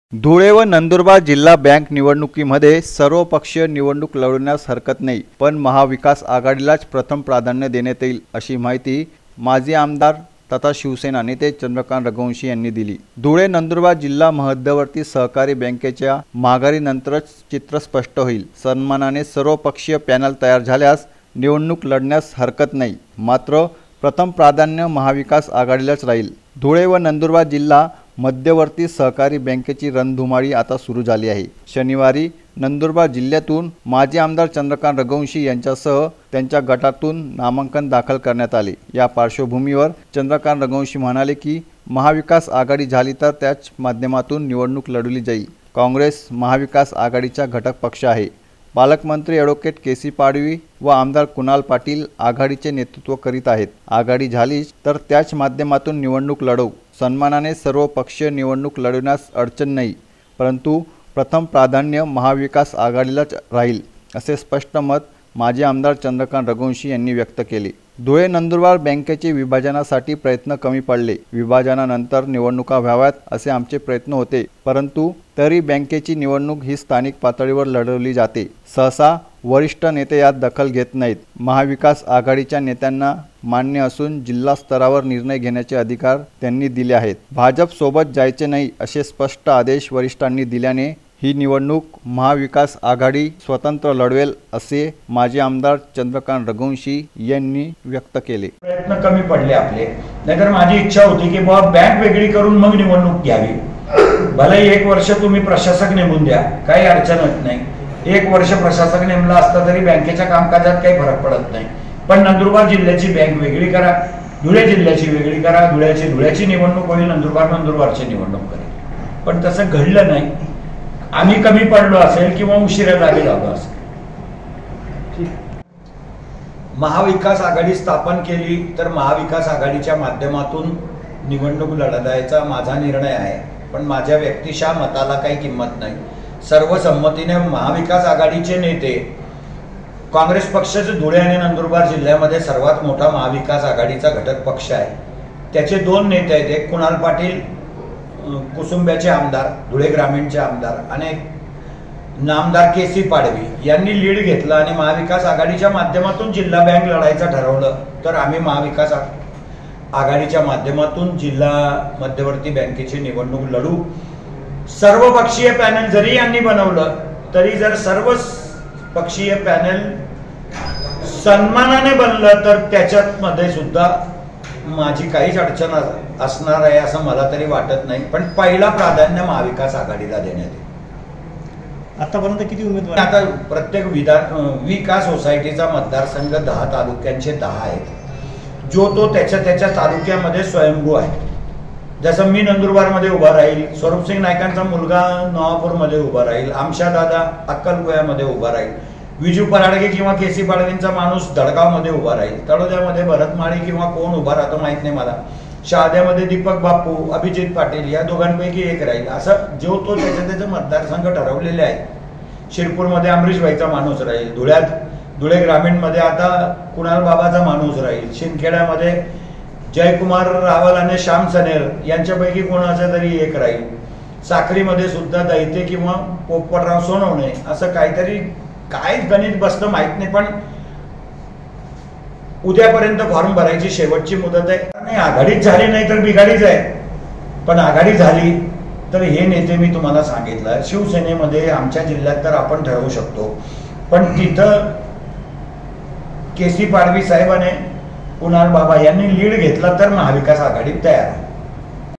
नंदुरबार | चंद्रकांत रघुवंशी |सर्वपक्षीय निवडणूक लढवण्यास हरकत नाही पण, महाविकास आघाडीलाच प्राधान्य
नंदुरबार- आमदार चंद्रकांत रघुवंशी यांचे सूतोवात.